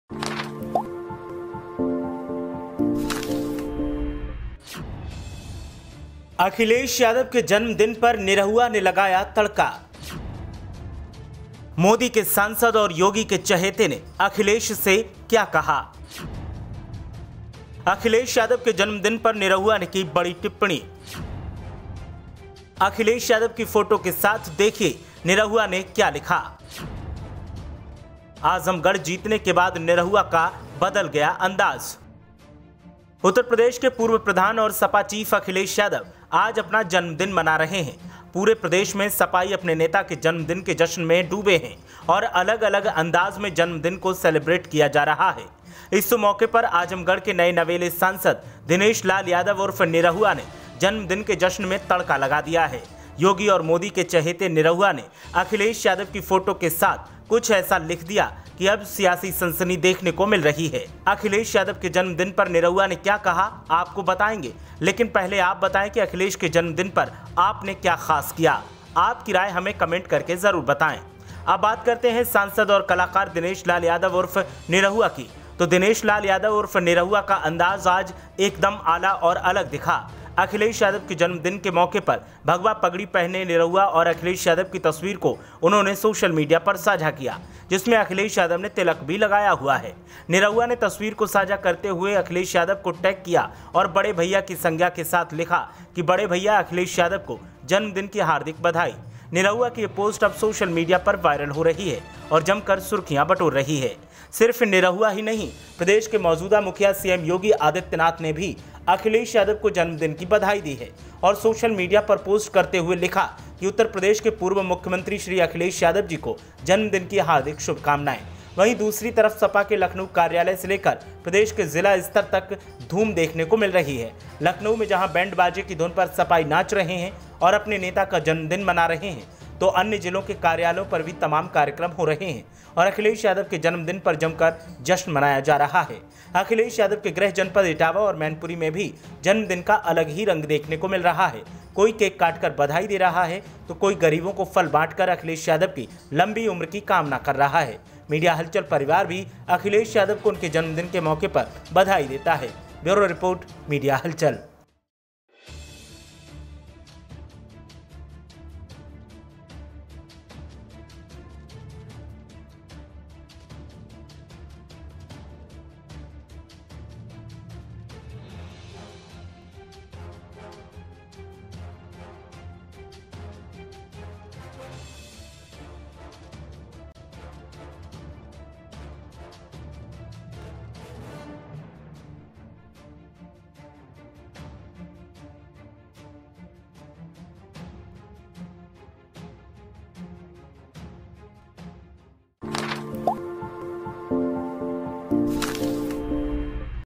सांसद बनते ही बदला निरहुआ का अंदाज II अखिलेश यादव के जश्न में हुए शरीक II पहना दी भगवा पगड़ी !
अखिलेश यादव के जन्मदिन पर निरहुआ ने लगाया तड़का !
मोदी के सांसद और योगी के चहेते ने अखिलेश से क्या कहा ?
अखिलेश यादव के जन्मदिन पर निरहुआ ने की बड़ी टिप्पणी !
अखिलेश की फोटो के साथ देखिए निरहुआ ने क्या लिखा ?
आजमगढ़ जीतने के बाद निरहुआ का बदल गया अंदाज !

उत्तर प्रदेश के पूर्व प्रधान और सपा चीफ अखिलेश यादव आज अपना जन्मदिन मना रहे हैं…पूरे प्रदेश में सपाई अपने नेता के जन्मदिन के जश्न में डूबे हैं…और अलग-अलग अंदाज में जन्मदिन को सेलेब्रेट किया जा रहा है…इस मौके पर आजमगढ़ के नए नवेले सांसद दिनेश लाल यादव उर्फ निरहुआ ने जन्मदिन के जश्न में तड़का लगा दिया है… योगी और मोदी के चहेते निरहुआ ने अखिलेश यादव की फोटो के साथ कुछ ऐसा लिख दिया कि अब सियासी सनसनी देखने को मिल रही है…अखिलेश यादव के जन्मदिन पर निरहुआ ने क्या कहा आपको बताएंगे लेकिन पहले आप बताएं कि अखिलेश यादव के जन्मदिन पर आपने क्या खास किया…आपकी राय हमें कमेंट करके जरूर बताएं…अब बात करते हैं सांसद और कलाकार दिनेश लाल यादव उर्फ निरहुआ की…तो दिनेश लाल यादव निरहुआ का अंदाज आज एक दम आला और अलग दिखा…अखिलेश यादव के जन्मदिन के मौके पर भगवा पगड़ी पहले निरहुआ और अखिलेश यादव की तस्वीर को सोशल मीडिया पर साझा किया…जिसमें अखिलेश यादव ने तिलक भी लगाया हुआ है…निरहुआ ने तस्वीर को साझा करते हुए अखिलेश यादव को टैग किया और बड़े भैया की संज्ञा के साथ लिखा कि बड़े भैया अखिलेश यादव को जन्मदिन की हार्दिक बधाई…निरहुआ की ये पोस्ट अब सोशल मीडिया पर वायरल हो रही है और जमकर सुर्खियां भी बन रहीं हैं…सिर्फ निरहुआ ही नहीं प्रदेश के मौजूदा मुखिया सीएम योगी आदित्यनाथ ने भी अखिलेश यादव को जन्मदिन की बधाई दी है…और सोशल मीडिया पर पोस्ट करते हुए लिखा कि …उत्तर प्रदेश के पूर्व मुख्यमंत्री श्री अखिलेश यादव जी को जन्मदिन की हार्दिक शुभकामनाएं…वहीं दूसरी तरफ सपा के लखनऊ कार्यालय से लेकर प्रदेश के हर जिलास्तर कार्यालय पर धूम देखने को मिल रही है…लखनऊ में जहां बैंड बाजे की धुन पर सपाई नाच रहे हैं और अपने नेता का जन्मदिन मना रहे हैं तो अन्य जिलों के कार्यालयों पर भी तमाम कार्यक्रम हो रहे हैं और अखिलेश यादव के जन्मदिन पर जमकर जश्न मनाया जा रहा है…अखिलेश यादव के ग्रह जनपद इटावा और मैनपुरी में भी जन्मदिन का अलग ही रंग देखने को मिल रहा है…कोई केक काटकर बधाई दे रहा है तो कोई गरीबों को फल बांटकर अखिलेश यादव की लंबी उम्र की कामना कर रहा है…मीडिया हलचल परिवार भी अखिलेश यादव को उनके जन्मदिन के मौके पर बधाई देता है…ब्यूरो रिपोर्ट